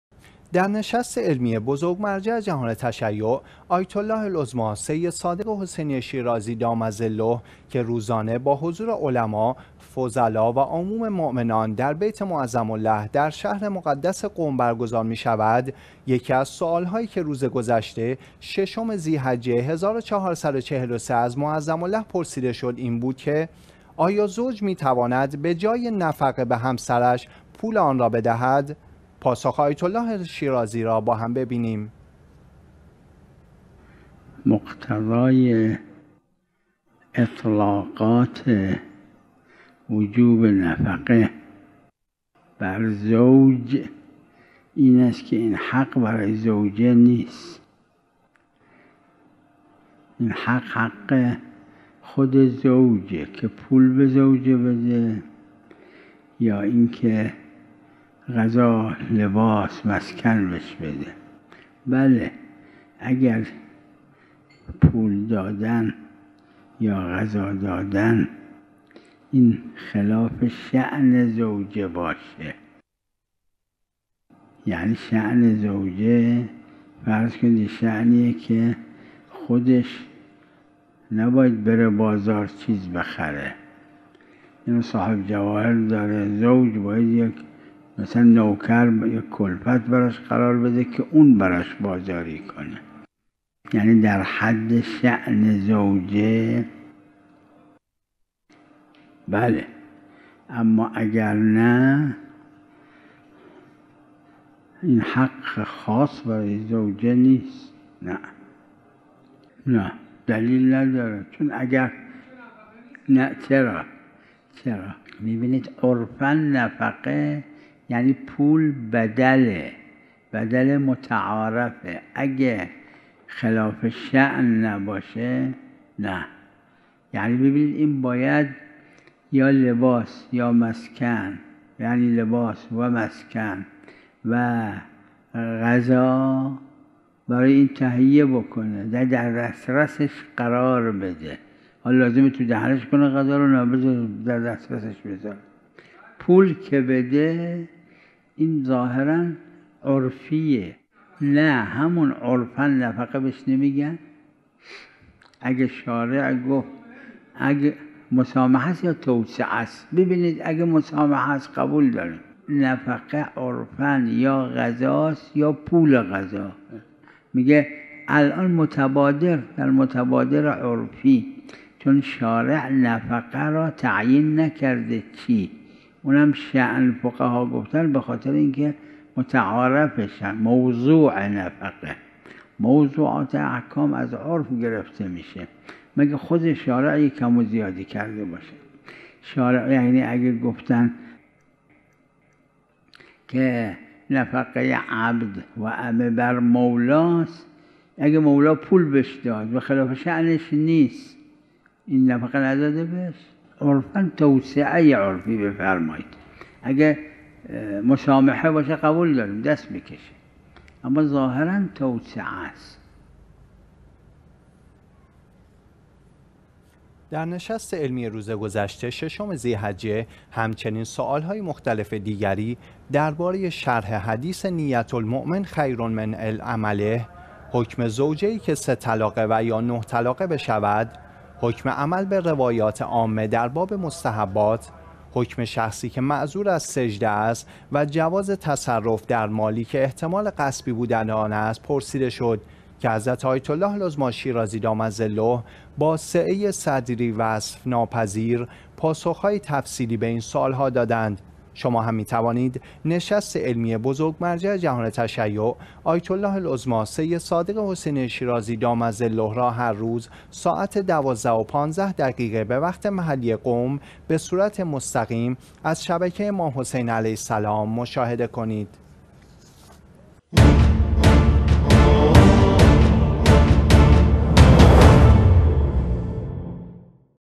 آیت الله العظمی شیرازی:بر زوج واجب است نفقه همسرش را متناسب با شأنيت او نقدی یا غیر نقدی بپردازد
در نشست علمی بزرگ مرجع جهان تشیع آیت الله العظمی سید صادق حسینی شیرازی دام ظله که روزانه با حضور علما ، فضلا و عموم مؤمنان در بیت معظم له در شهر مقدس قم برگزار می‌شود ، یکی از سؤال‌هایی که روز گذشته ششم ذی حجه ۱۴۴۳ از معظم له پرسیده شد این بود که:

آيا زوج مى‌تواند به جاى نفقه به همسرش ، پول آن را بدهد؟

آیت الله العظمی شیرازی در پاسخ فرمودند:

 (برطبق روايات، آنچه كه بر زوج واجب است اين است كه براى همسرش لباس و خوراك و مسكن در حد شأن او تهيه كند يا پول اين ها را به زوجه بدهد.)

در نشست علمی روز گذشته ، ششم ذی حجه همچنین سؤال‌های مختلف دیگری درباره شرح حديث: نيتُ المؤمن خيرٌ مِن عملِه، حكم زوجه‌اى كه سه طلاقه و يا نُه طلاقه بشود، حكم عمل به روايات عامه در باب مستحبات، حكم شخصى كه معذور از سجده است و جواز تصرف در مالى كه احتمال غصبى بودن آن است، پرسیده شد که حضرت آیت الله العظمی شیرازی دام ظله با سعه صدری وصف‌ناپذیر پاسخ‌های تفصیلی به این سؤال‌ها دادند.